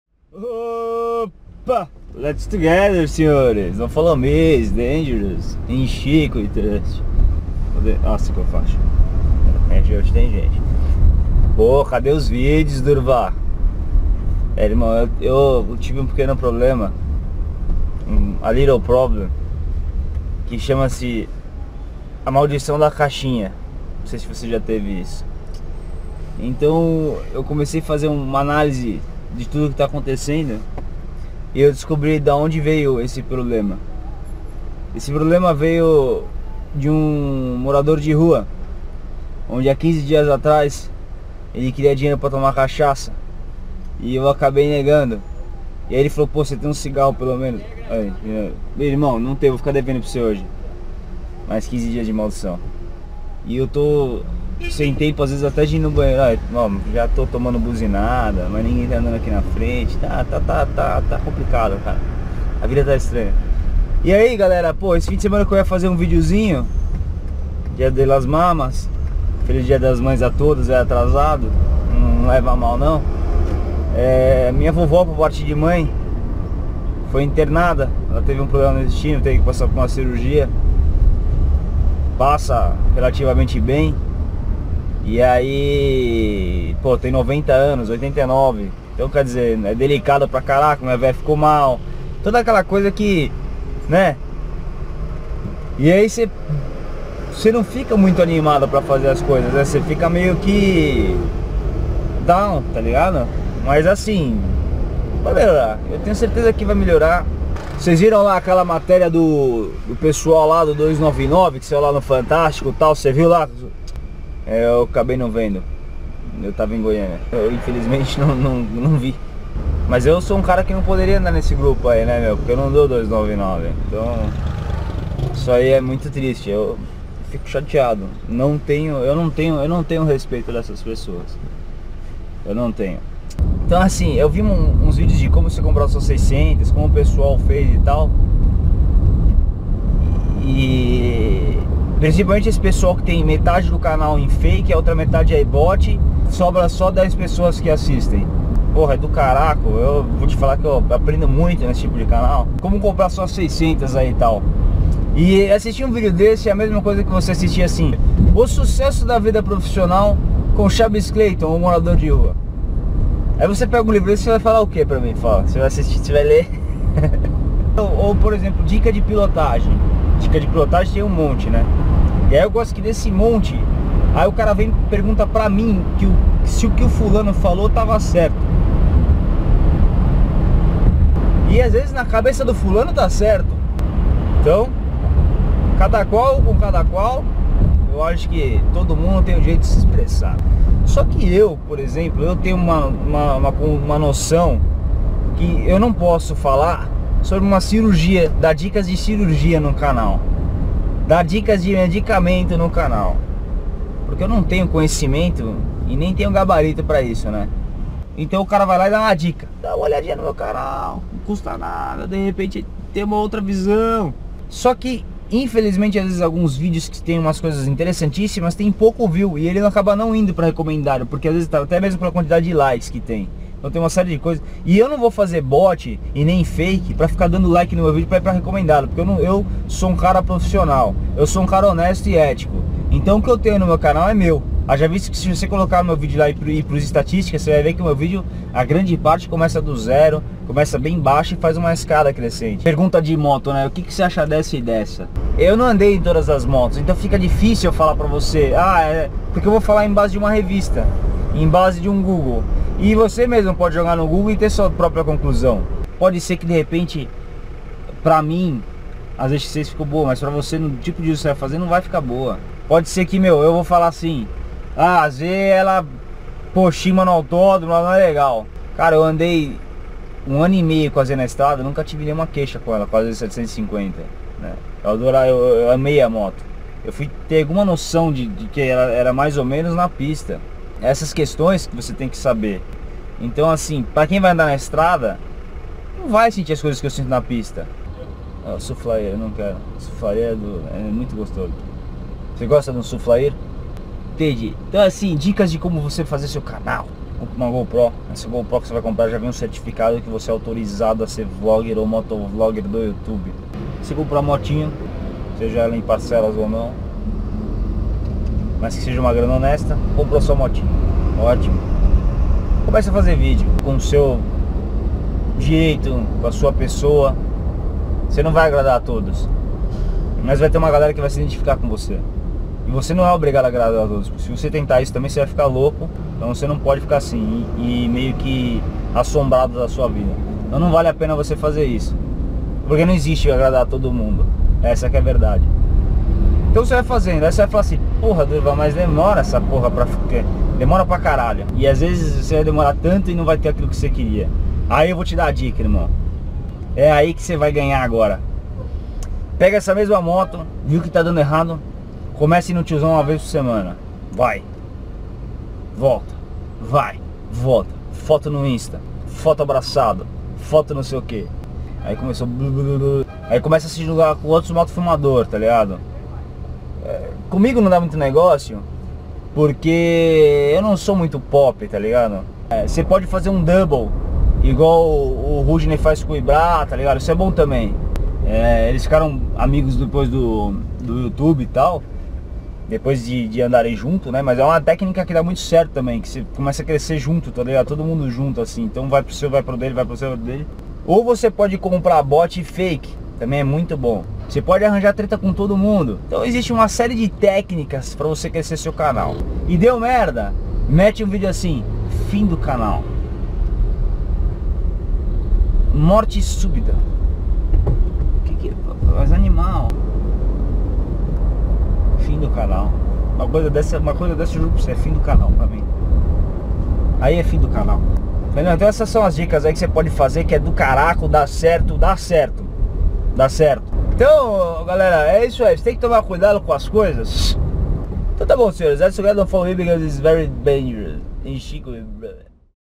I'm Here Fiiilho com assuntos de montinho 5 - Durvalcareca
Um mix de coisa phyna para você, desde minha correria, pancadas da vida, 299, coisas que vejo no youtube, fakes and boots, cuidado com as dicas alheias e como ter um canal de sucesso de forma rápida, fácil e segura auuuuuuuuu.

Vejo que diante de tanta coisa, não podemos parar afinal somos o equilíbrio das coisas sensatas. Olha esso seu hardcore

Grande abraxxx.